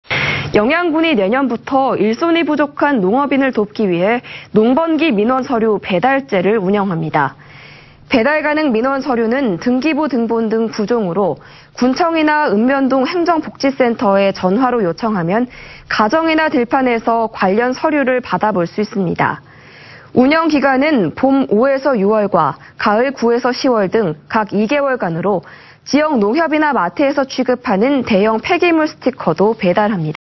영양군, 농번기 민원서류 배달제 운영/ 안동MBC
2020/12/27 10:00:11    작성자 : 이호영

영양군이 내년(2021)부터
일손이 부족한 농업인을 돕기 위해
'농번기 민원서류 배달제'를 운영합니다.
◀ＥＮＤ▶

배달 가능 민원서류는
등기부등본 등 9종으로 군청이나
읍면동 행정복지센터에 전화로 요청하면
가정이나 들판에서 관련 서류를 받아볼 수
있습니다.

운영 기간은 봄 5~6월과 가을 9~10월 등
각 2개월간으로 지역 농협이나 마트에서
취급하는 대형 폐기물스티커도 배달합니다.